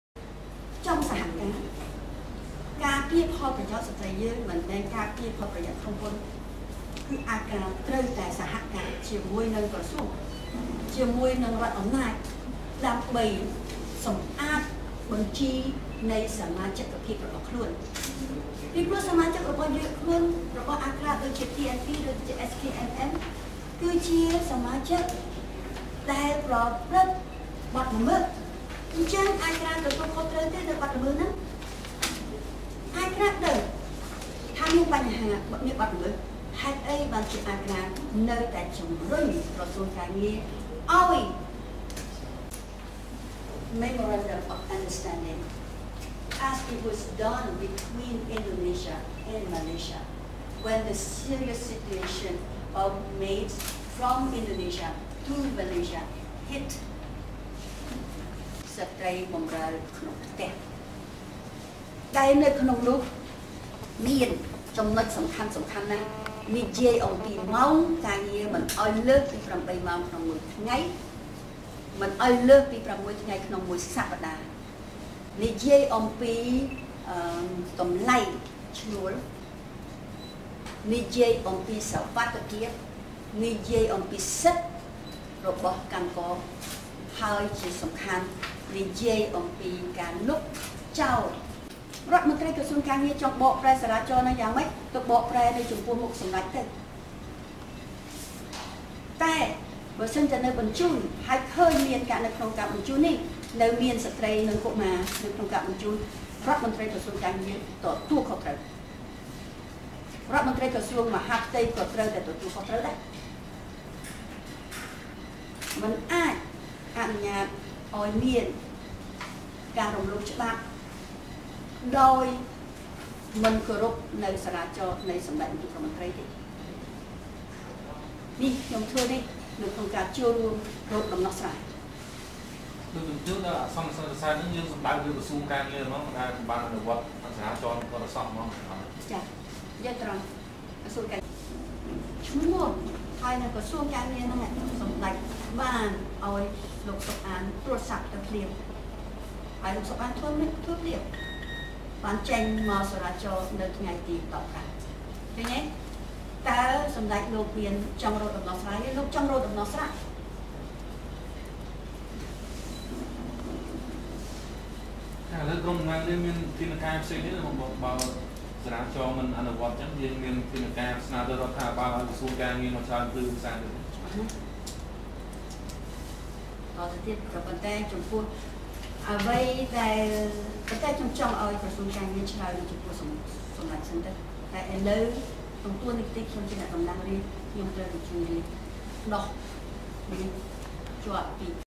លោកស្រី មូ សុខហួរ
លោកស្រី មួ សុខហួរ ចង់អោយក្រសួងការងារបកស្រាយចំពោះសារាចររបស់រដ្ឋាភិបាល ក្នុងការអោយផ្អាកនាំពលករទៅធ្វើការនៅប្រទេសមាឡេស៊ី